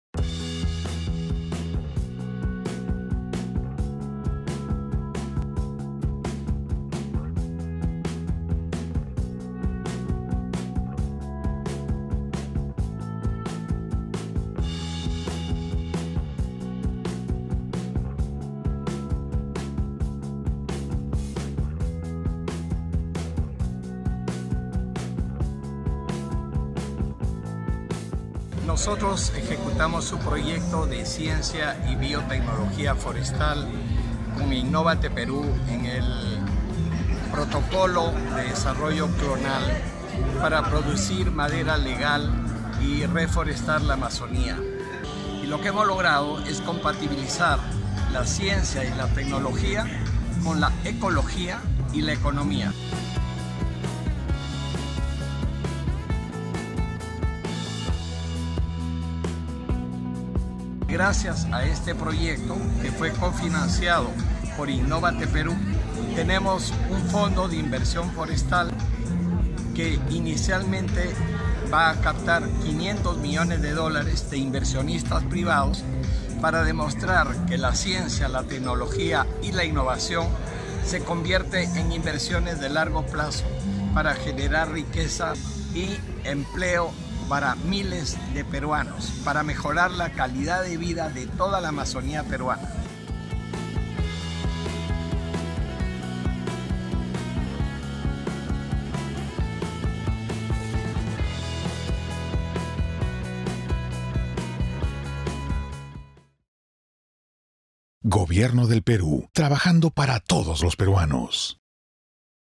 Innóvate Perú | Entrevista a Enrique Toledo, gerente de Reforesta Perú
La empresa nacional Reforesta Perú obtuvo el reconocimiento especial del año "Mérito a la Innovación: Innóvate 2017", otorgado por el Ministerio de la Producción (PRODUCE), a través de Innóvate Perú, por promover la recuperación y puesta en valor de las tierras degradadas de la amazonia peruana, a través de las plantaciones forestales clonales.

Reforesta Perú recibió el cofinanciamiento de Innóvate Perú para desarrollar protocolos para la producción de plantones clonales de siete especies maderables nativas amazónicas, como la caoba, cedro, tornillo, capirona, marupa, estoraque, quinilla, en base a semilla vegetativa de árboles plus en la Región San Martín.

Lima, diciembre de 2017